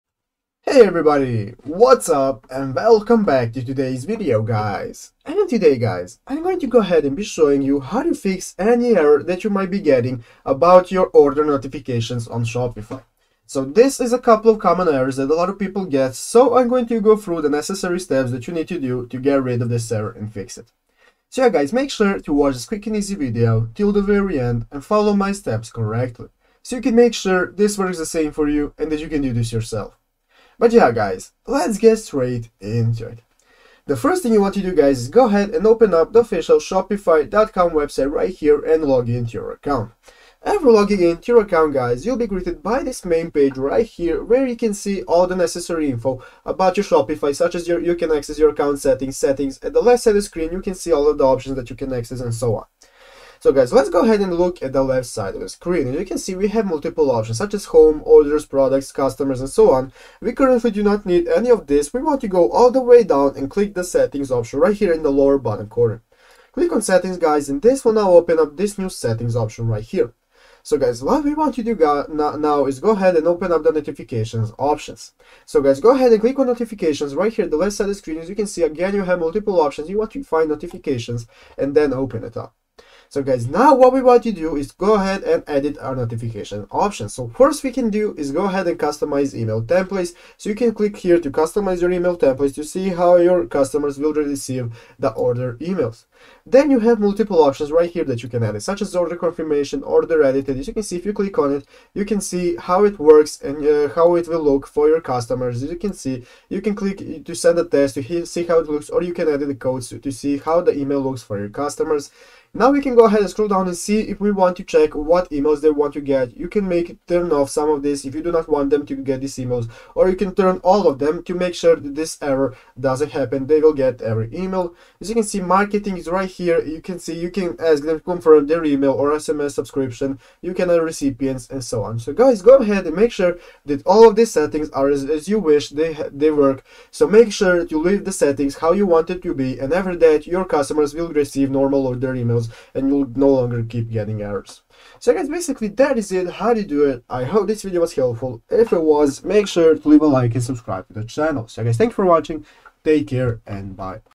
How To Fix Shopify Error With Order Notification Emails
Today we talk about shopify error with order notification emails,shopify email notifications,shopify tutorial,shopify email,shopify order notifications